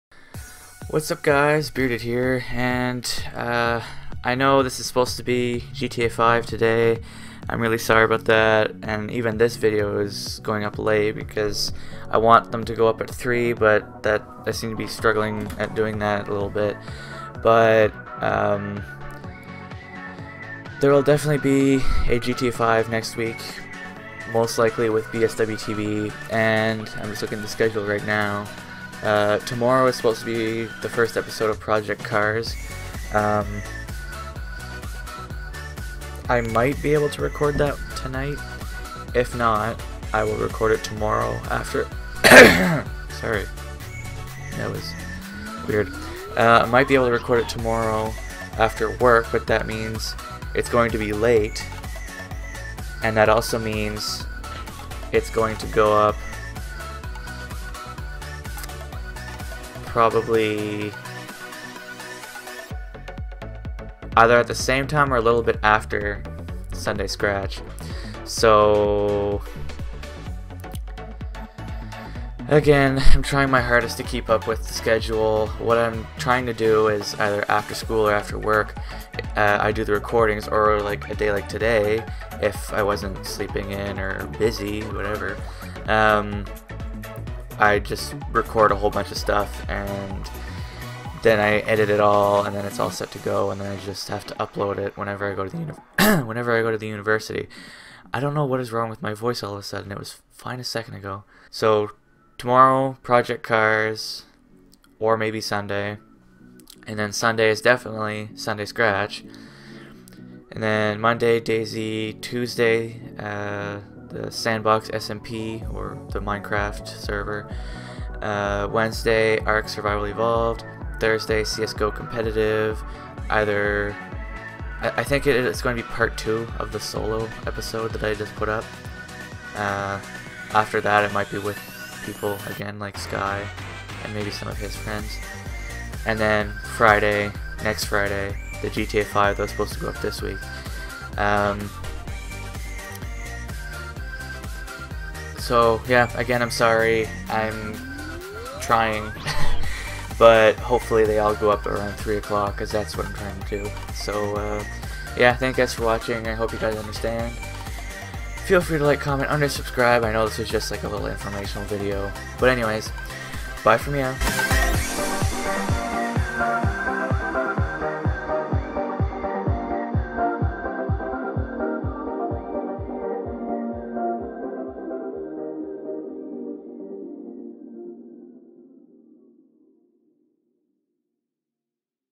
No GTA V This Week (Sorry) :(
I just can't do it this week because one i'm a little too busy and two BSWTB couldn't be with me for the recording due to me being busy...